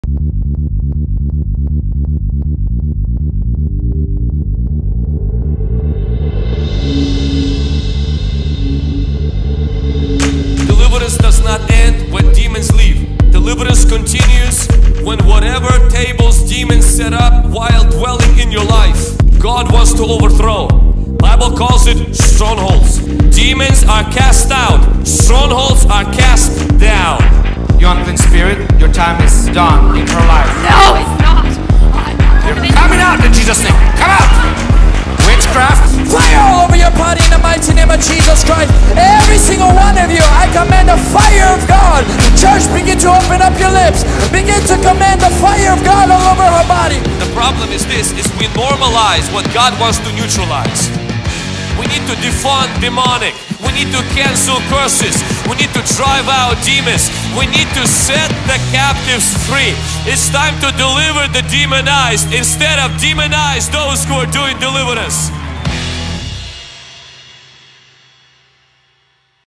Raised to Deliver 2021 | Vancouver WA | OFFICIAL TRAILER
Our annual Fall 2021 conference is right around the corner we will be hosting it in Vancouver, WA. We are excited to invite you:

Location:
7250 NE 41st St, Vancouver, WA 98662
Church of Truth

Ministers: @vladhungrygen, Ilya Parkhotyuk, Zack Parkhotyuk

Schedule:
*Thursday
4pm-7pm Pre-Prayer Counseling
*Friday
2pm-4pm Pre-Prayer Counseling
6pm Evening Service
*Saturday
10am-12pm Morning Session
2pm-5pm Pre-Prayer Counseling
6pm Evening Service
*Sunday
6pm Final Service

Nearest ✈️ airport: Portland International Airport, 7000 NE Airport Way, Portland, OR 97218

DELIVERANCE RESOURCES
Need Deliverance?

Follow @vladhungrygen YouTube for more deliverance content.
Follow @rikhardhart on YouTube for more deliverance content.